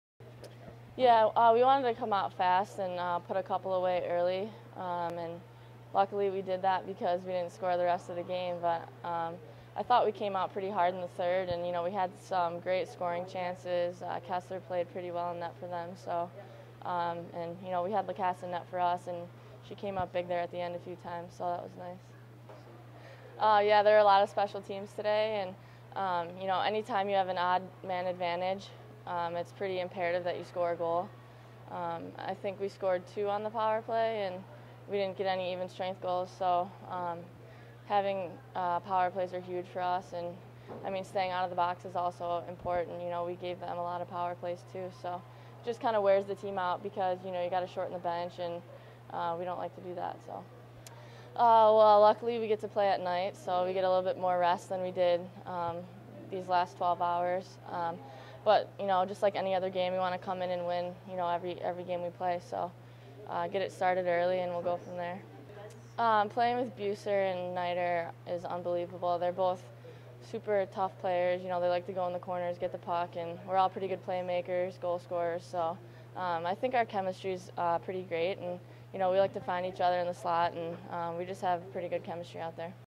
CWHL Clarkson Cup - Day 2 - Post Game Interview with Boston Blade's Kelli Stack
Our post-game interview with Boston Blade's and Team USA's Kelli Stack after their day 2 victory over the Toronto Furies.